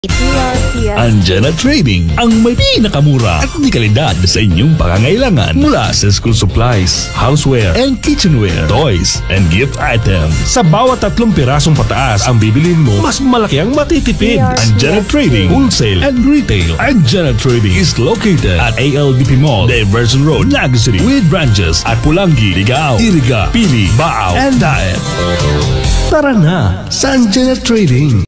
Angena Trading Radio Ad 2024 28s (Naga) [ST]
Ito po ang 2024 Provincial Radio Ad ng Angena Trading sa Naga.

DRPS Two 6979: Angena Trading Radio Ad 2024 28s (Naga) [ST]